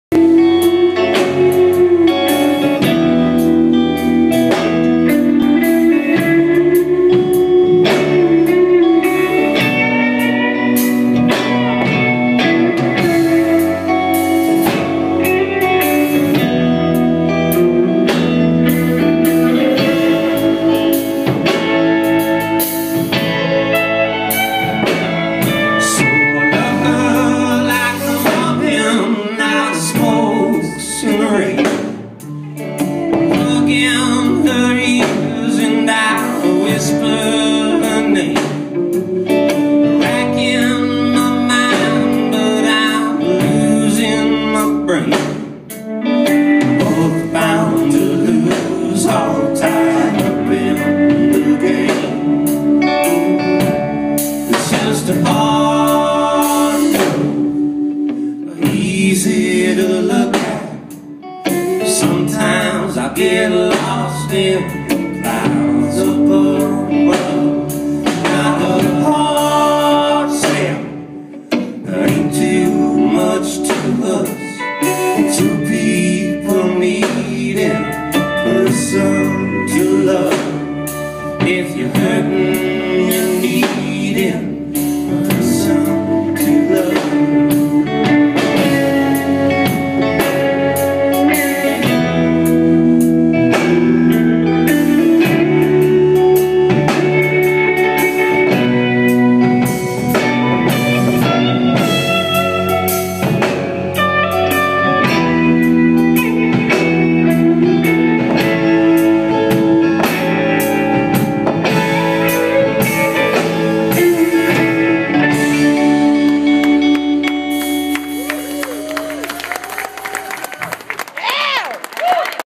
The Union Suits
The Union Suits are a badass, outlaw country band from Minneapolis. Everyone should like them. Trust me